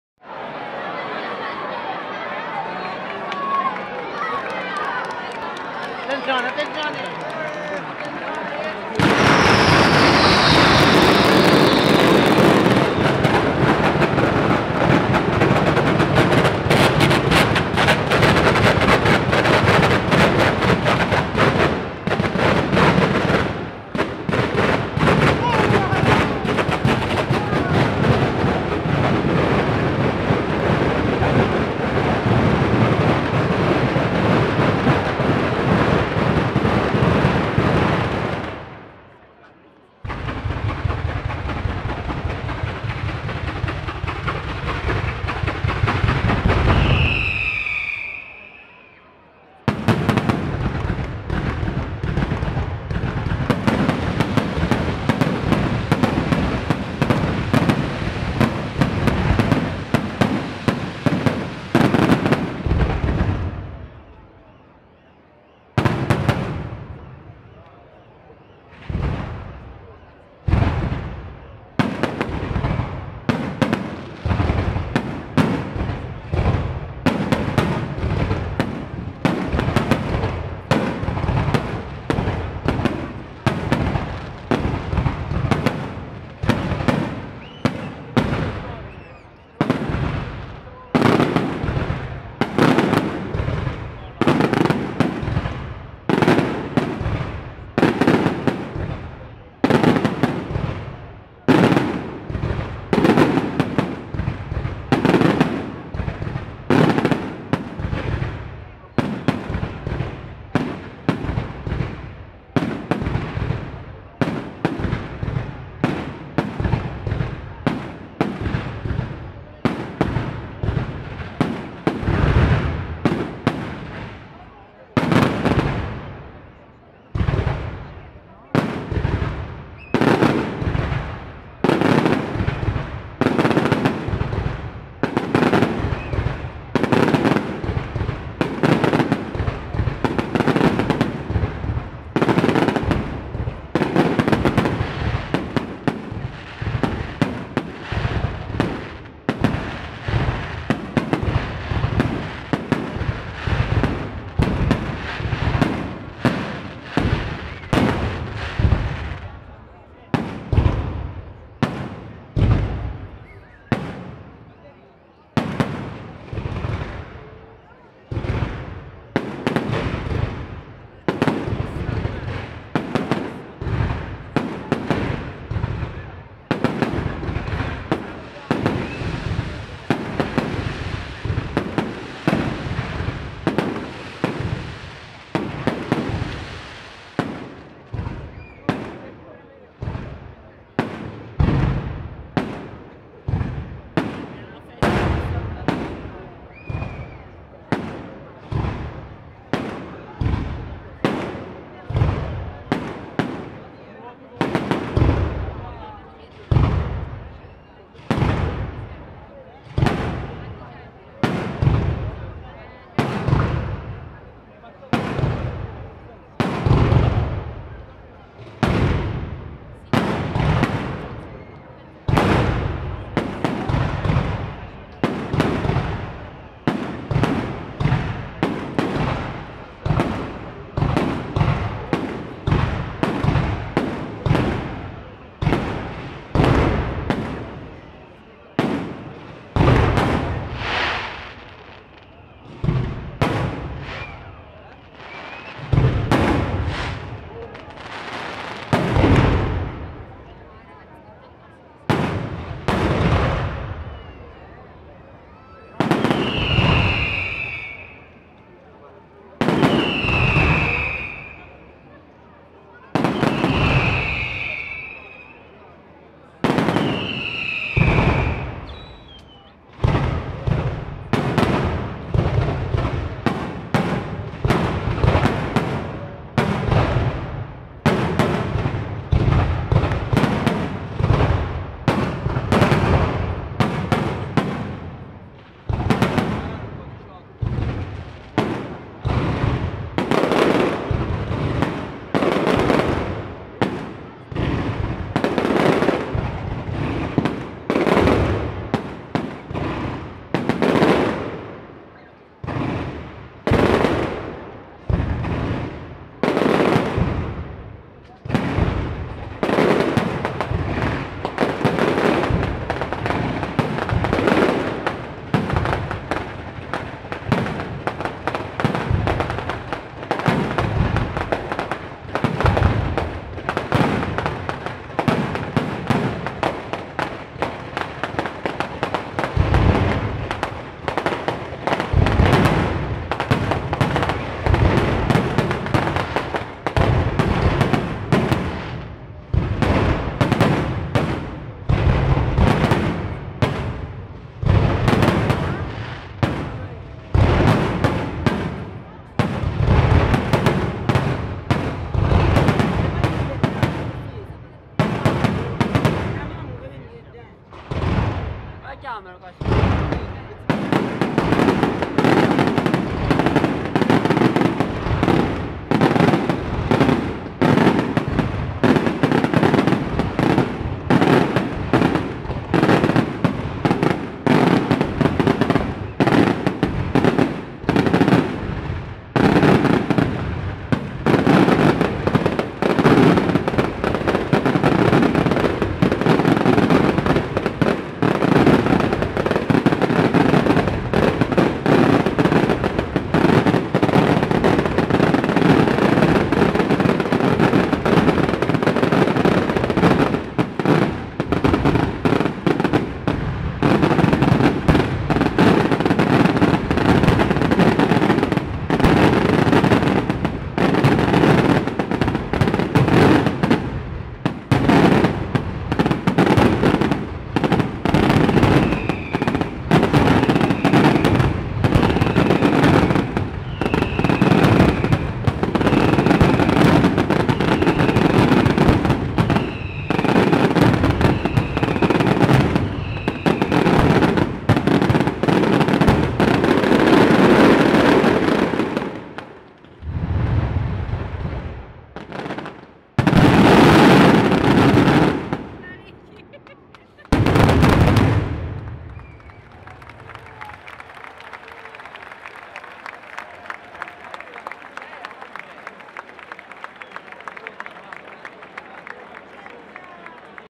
ACICATENA,15 AGOSTO 2016 - FESTA DI MARIA SS. DELLA CATENA
"TRASUTA O CHIANU" con spettacolo pirotecnico conclusivo della ditta "ALESSANDRO SPINA"